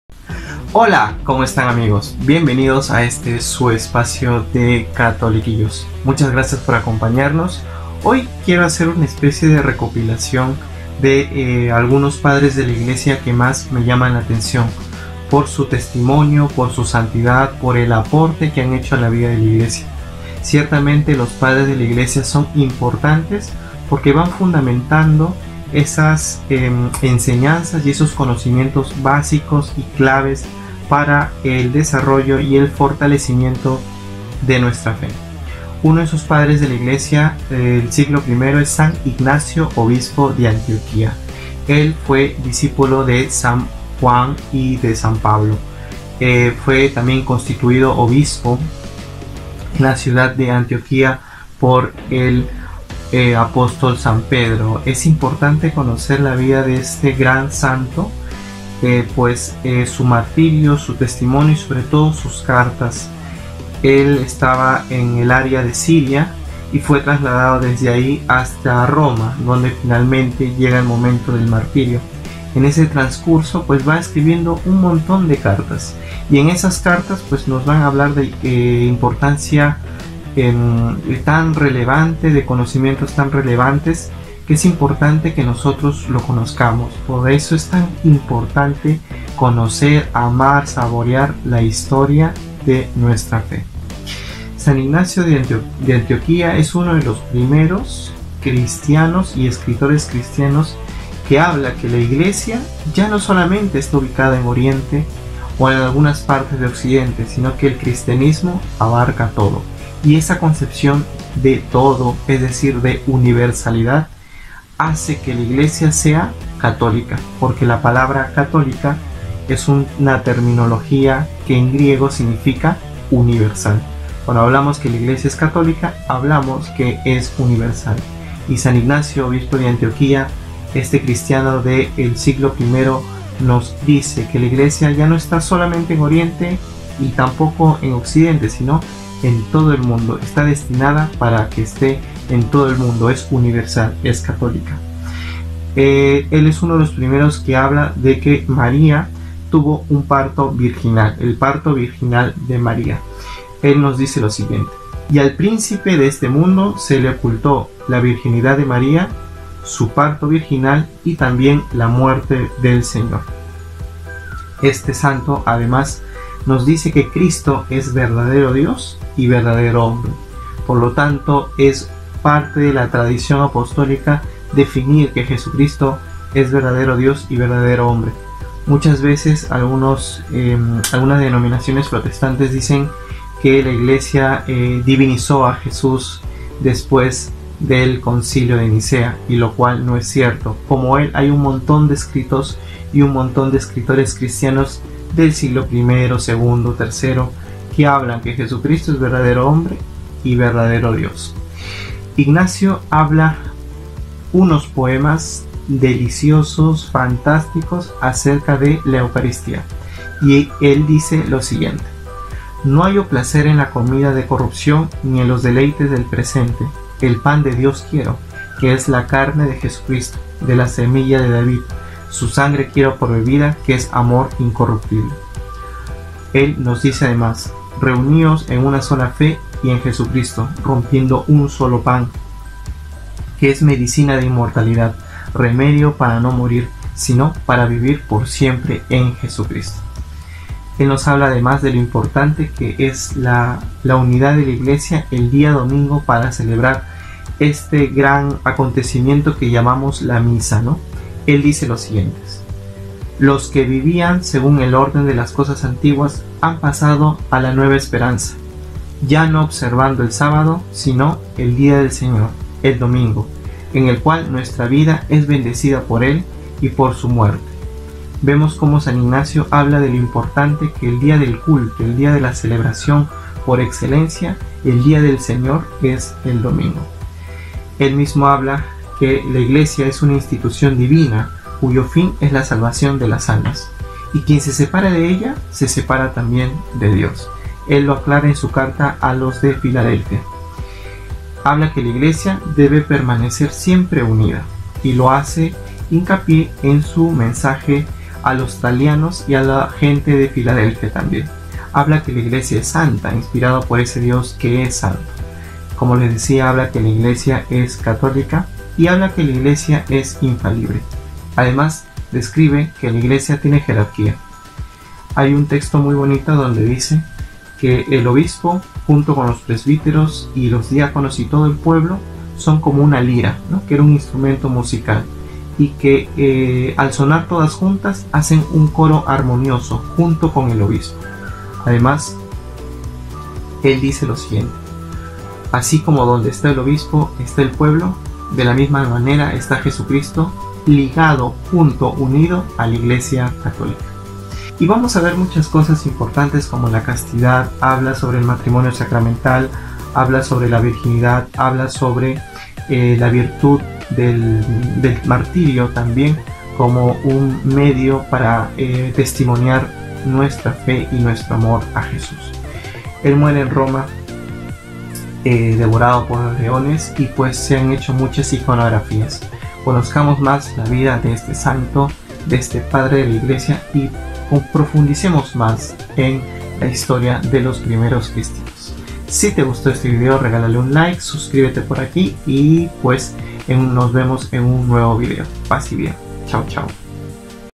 SAN IGNACIO DE ANTIOQUÍA / PRIMEROS CRITIANOS / ORÍGENES
Les compartimos una breve reseña de las enseñanzas de Ignacio, Obispo de Antioquia, un cristiano del siglo I .